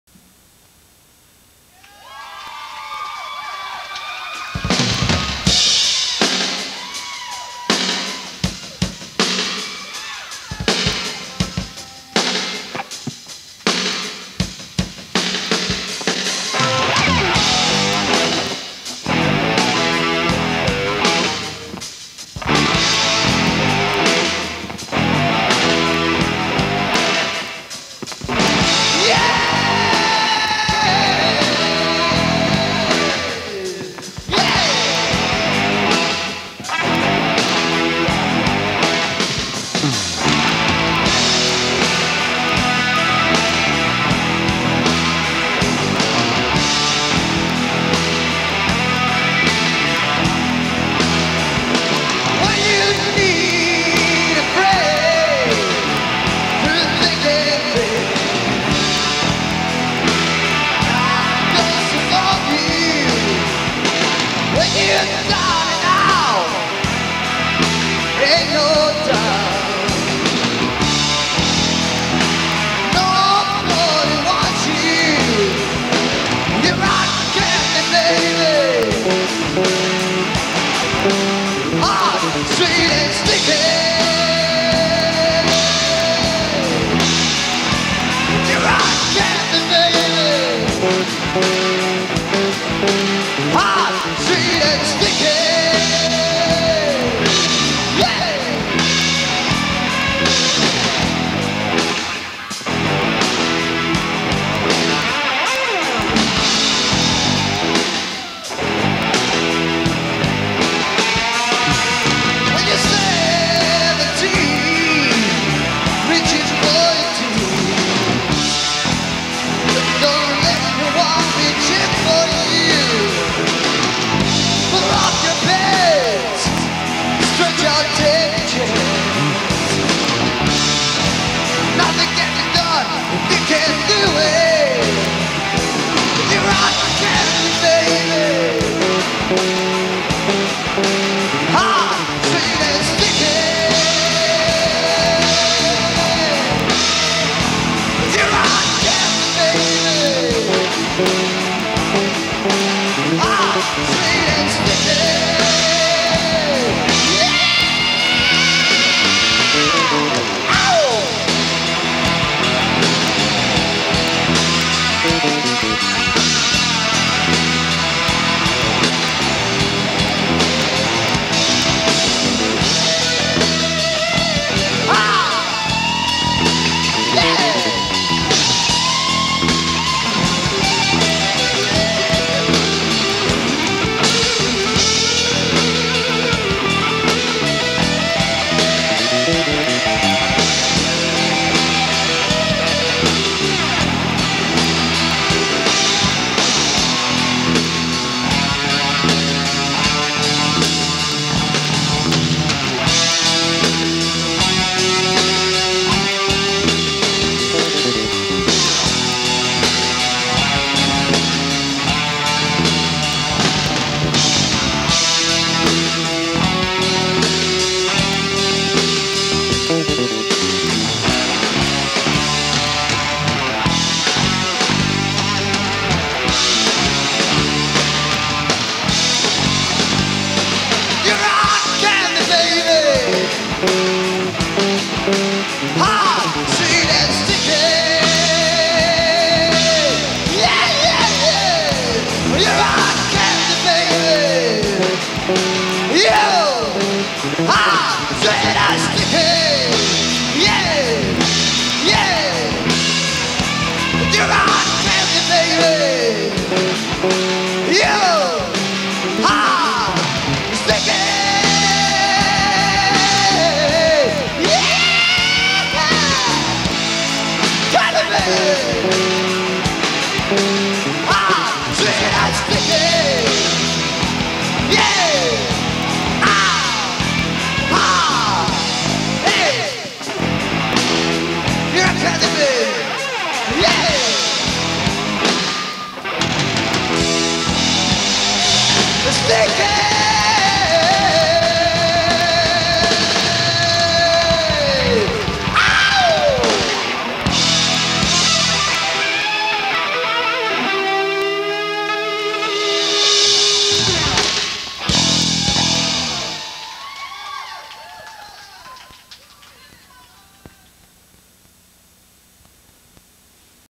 Rock Candy LIVE at GIT
Glenn Jepsen, Hal Abrams, Adolph Bringas, Terry Law and Glen Gorowsky play live at GIT in Los Angeles. 3/15/1986